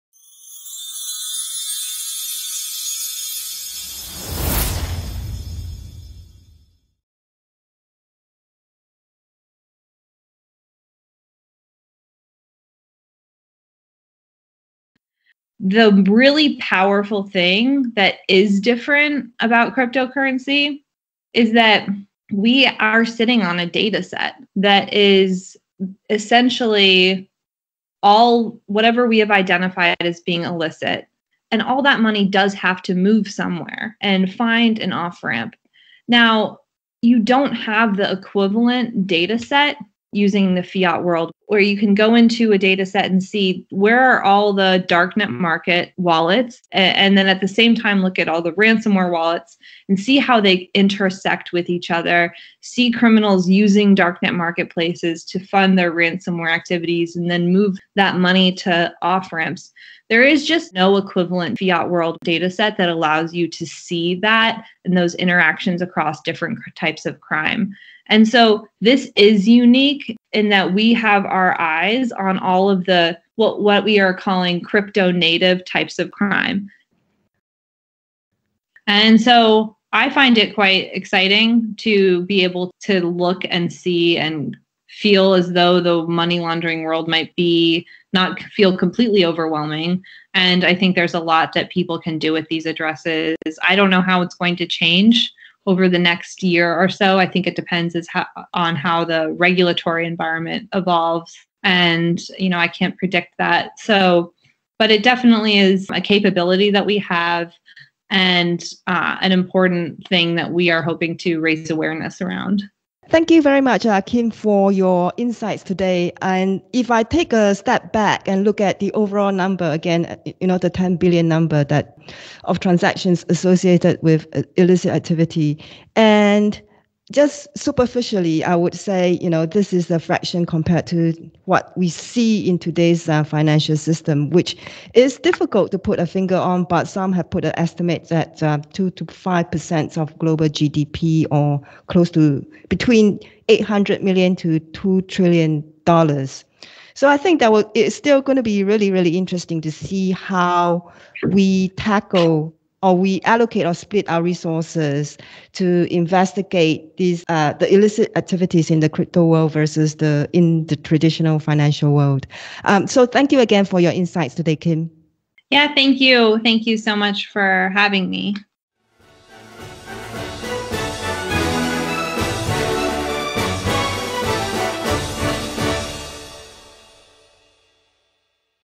The Big Biz of Crypto-crime [Podcast clip]
Kim Grauer is the Head of Research at Chainalysis, where she examines trends in cryptocurrency economics and crime.  Using Chainalysis data and software, she works with government agencies, exchanges, financial institutions, and insurance and cybersecurity companies to help solve some of the world’s most high-profile cyber criminal cases.

Kim was trained in economics at the London School of Economics and in politics at Oxford University. Previously, she explored technological advancements in developing countries as an academic research associate at the London School of Economics and was an economics researcher at the New York City Economic Development Corporation.

In this podcast, Jane Lo, Singapore Correspondent interviews Kim to highlight key findings from the Crypto Crime Report 2021 and the criminal activities that contribute to USD 10 billion of illicit transactions in crypto currency during 2020.

She introduces the approach to identify and map illicit transactions to crypto currency wallet addresses, and the methodology to trace the crypto proceeds from crimes, to their placement into today’s financial system.

She also discusses the outlook for the top crime types transacted in crypto currency – Scams, Darknet Markets, and Ransomware, and the trends observed year-on-year.

Drawing on distinctions in data and information available in the fiat versus the crypto world, she shares some key takeaways for law enforcement and anti-money laundering specialists.

▶Recorded 25th February 2021 (7am Singapore /6pm [-1] New York)